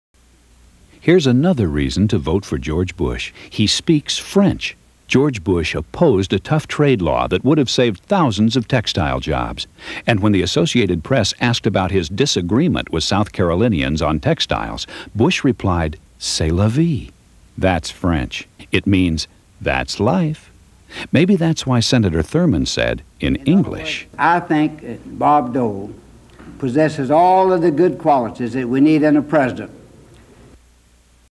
1988 Bob Dole for President TV Ad #6 - "Bush speaks French!"
1988 Bob Dole for President TV Ad #6 - "Bush speaks French!"